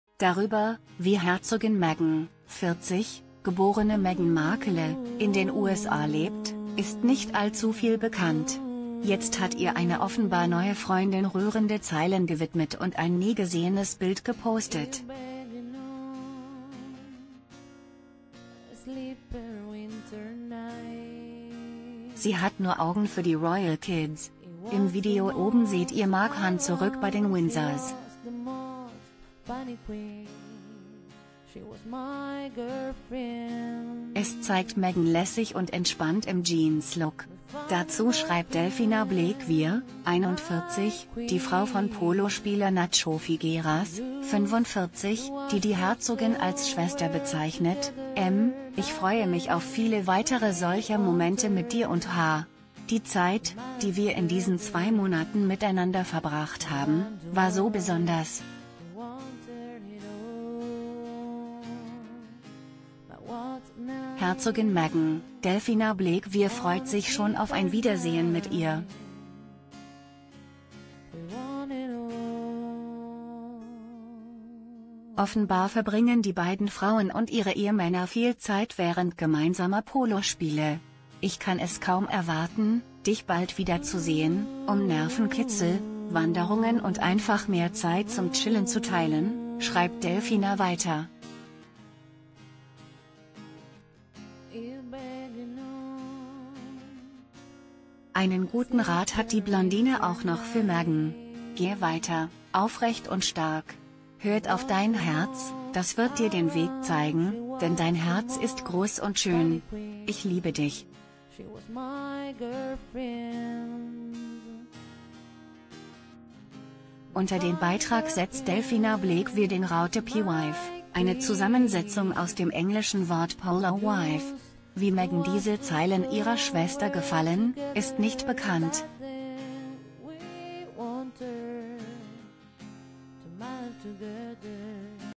Herzogin Meghan : "Meine Schwester": Neue Freundin zeigt nie gesehenes Bild von ihr
Darüber, wie Herzogin Meghan (40, geborene Meghan Markle) in den USA lebt, ist nicht allzu viel bekannt. Jetzt hat ihr eine offenbar neue Freundin rührende Zeilen gewidmet und ein nie gesehenes Bild gepostet.
Sie hat nur Augen für die Royal-Kids! Im Video oben seht ihr Maghan zurück bei den Windsors
Es zeigt Meghan lässig und entspannt im Jeanslook. Dazu schreibt Delfina Blaquier (41), die Frau von Polospieler Nacho Figueras (45), die die Herzogin als "Schwester" bezeichnet: "M – ich freue m